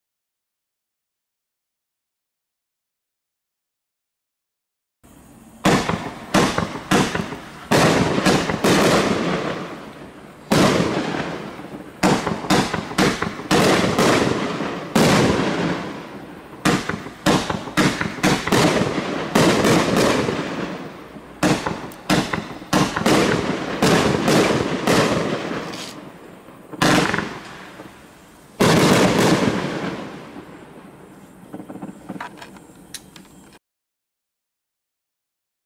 SW C2199 NEVER RETREAT NEVER SURRENDER
1.2'' 20s Gold coco to green strobe mine blast gold coco to red ,gold coco to green ,gold glittering willow tail blast green gold strobe